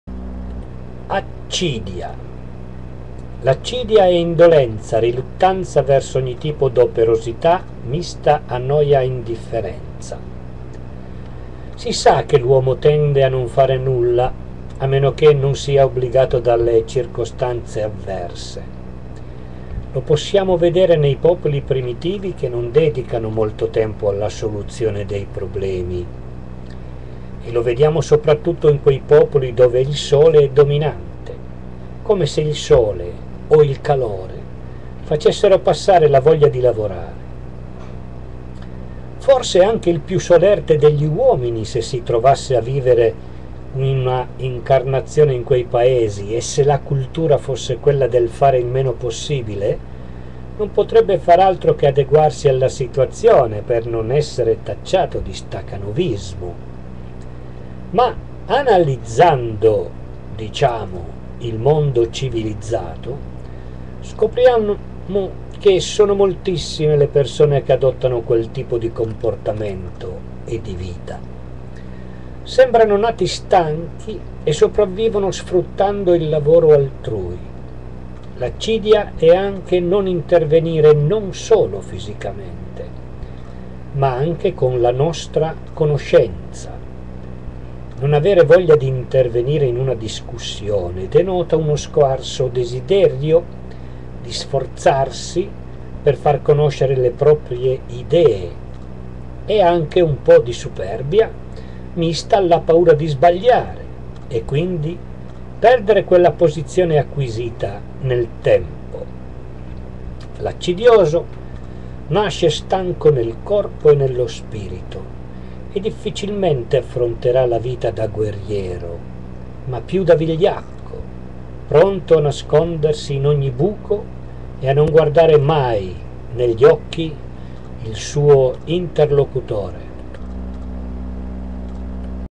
I SETTE PECCATI CAPITALI ACCIDIA
Si sa che l'uomo tende a non fare nulla ameno che non sia obbligato dalle circostanze avverse. Lo possiamo vedere nei popoli primitivi che non dedicano molto tempo alla soluzione dei problemi. E lo vediamo soprattutto in quei popoli dove il sole è dominante...come se il sole o il calore facessero passare la voglia di lavorare. Forse anche il più solerte degli uomini se si trovasse a vivere una incarnazione in quei paesi e se la cultura fosse quella del fare il meno possibile. non potrebbe far altro che adeguarsi alla situazione per non essere tacciato di STACANOVISMO. Ma analizzando diciamo il mondo civilizzato scopriamo che sono moltissime le persone che adottano quel tipo di comportamento e di vita. Sembrano nati stanchi e sopravvivono sfruttando il lavoro altrui. L'ACCIDIA è anche non intervenire non solo fisicamente ma anche con la nostra conoscenza. Non avere voglia di intervenire in una discussione denota uno scarso desiderio di sforzarsi per far conoscere le proprie idee e anche un po' di SUPERBIA mista alla paura di sbagliare e quindi perdere quella posizione acquisita nel tempo. L'ACCIDIOSO nasce stanco nel corpo e nello SPIRITO e difficilmente affronterà la vita da GUERRIERO ma più da VIGLIACCO pronto a nascondersi in ogni buco e a non guardare mai negli occhi il suo interlocutore.